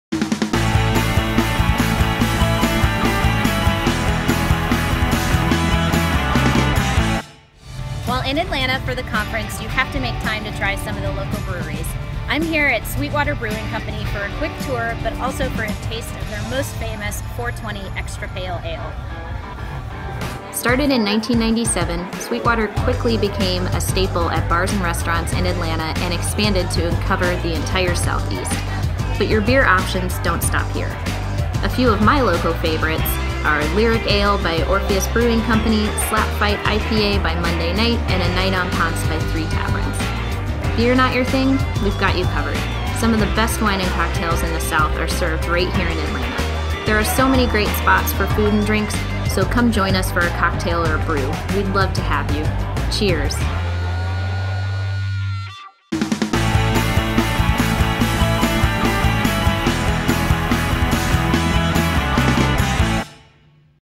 NAA's Tour Atlanta: Check Out These Local Watering Holes
Join Erin from AMLI Residential as she reveals to the NAA Network all the great brewers in Atlanta, just one way to spend time while you're in town for the apartment industry's largest, most important annual event: The NAA Education Conference & Exposition, June 21-24 at the Georgia World Congress Center. Register today at educonf.naahq.org